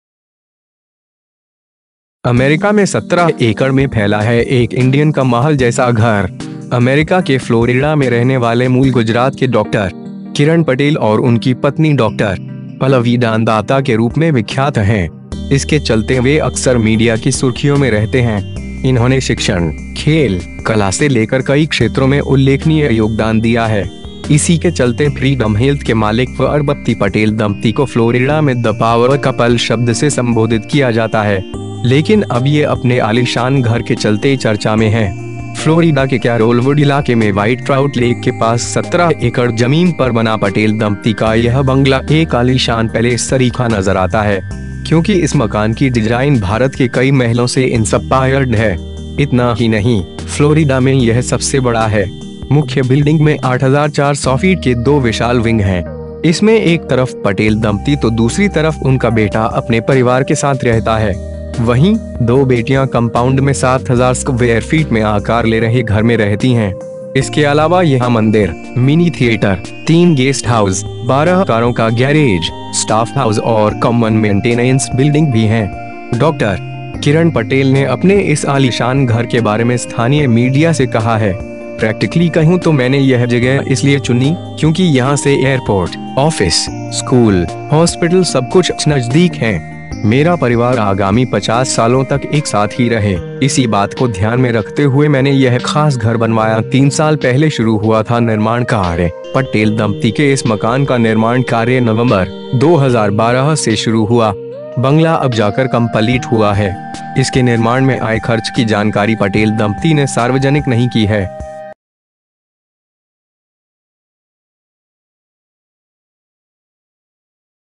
अमेरिका में 17 एकड़ में फैला है एक इंडियन का महल जैसा घर,
अमेरिका के फ्लोरिडा में रहने वाले मूल गुजरात के डॉ. किरण पटेल और उनकी पत्नी डॉ. पल्लवी दानदाता के रूप में विख्यात हैं। इसके चलते वे अक्सर मीडिया की सुर्खियों में रहते हैं। इन्होंने शिक्षण, खेल, कला से लेकर कई क्षेत्रों में उल्लेखनीय योगदान दिया है। इसी के चलते फ्रीडम हेल्थ के मालिक व अरबपति पटेल दंपती को फ्लोरिडा में ‘द पावर कपल’ शब्द से संबोधित किया जाता है। लेकिन अब ये अपने आलीशान घर के चलते चर्चा में हैं।

फ्लोरिडा के कैरोलवुड इलाके में व्हाइट ट्राउट लेक के पास 17 एकड़ जमीन पर बना पटेल दंपती का यह बंगला एक आलीशान पैलेस सरीखा नजर आता है। क्योंकि इस मकान की डिजाइन भारत के कई महलों से इन्सपायर्ड है। इतना ही नहीं, फ्लोरिडा में यह सबसे बड़ा है।
मुख्य बिल्डिंग में 8400 फीट के दो विशाल विंग हैं। इसमें एक तरफ पटेल दंपती तो दूसरी तरफ उनका बेटा अपने परिवार के साथ रहता है। वहीं, दो बेटियां कंपाउंड में 7000 स्क्वेयर फीट में आकार ले रहे घर में रहती हैं। इसके अलावा यहां मंदिर, मिनी थिएटर, तीन गेस्ट हाउस, 12 कारों का गैरेज, स्टाफ हाउस और कॉमन मेंटेनेंस बिल्डिंग भी हैं।
डॉ. किरण पटेल ने अपने इस आलीशान घर के बारे में स्थानीय मीडिया से कहा है.. ‘प्रैक्टिकली कहूं तो मैंने यह जगह इसलिए चुनी, क्योंकि यहां से एयरपोर्ट, ऑफिस, स्कूल, हॉस्पिटल सबकुछ नजदीक हैं। मेरा परिवार आगामी 50 सालों तक एक साथ ही रहे, इसी बात को ध्यान में रखते हुए मैंने यह खास घर बनवाया।’
तीन साल पहले शुरू हुआ था निर्माण कार्य:
पटेल दंपती के इस मकान का निर्माण कार्य नवंबर, 2012 से शुरू हुआ। बंगला अब जाकर कम्पलीट हुआ है। इसके निर्माण में आए खर्च की जानकारी पटेल दंपती ने सार्वजनिक नहीं की है।